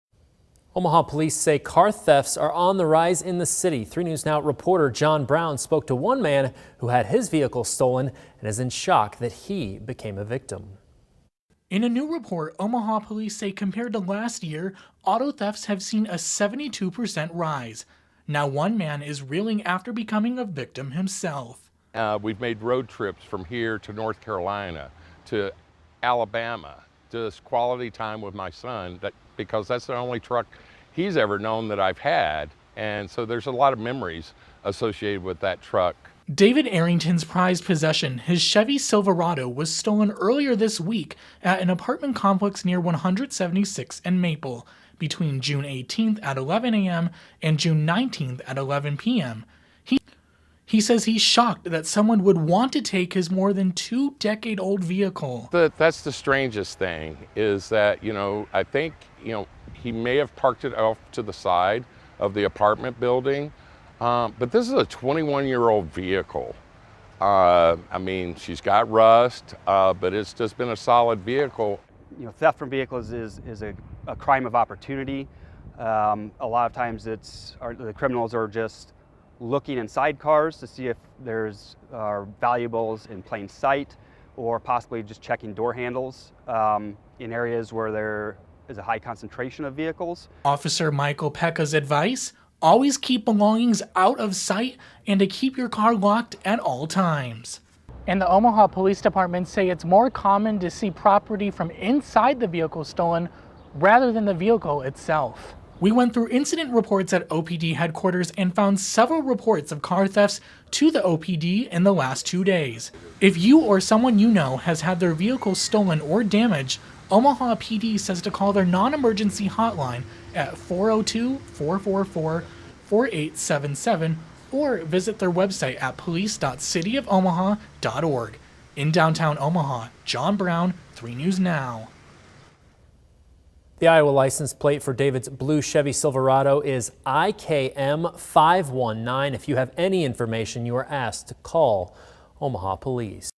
Omaha Police offer tips on how to protect vehicles as auto theft increases
Omaha Police said car thefts are on the rise in the city. In a new report, police said that auto thefts have seen a 72% rise compared to last year.